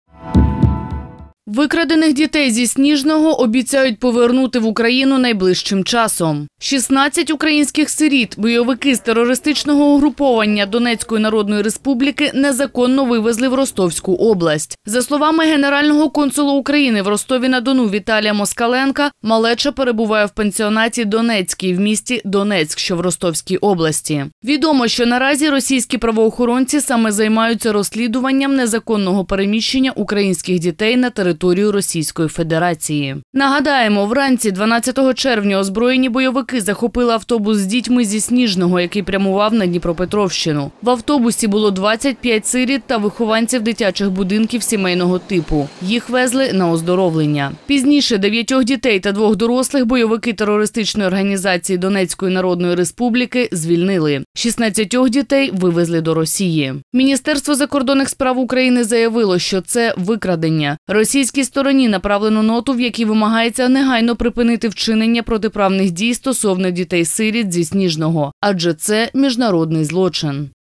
13.06. Викрадених Укаїнських Дітей Обіцяють Повернути.
13.06. Викрадених Укаїнських Дітей Обіцяють Повернути.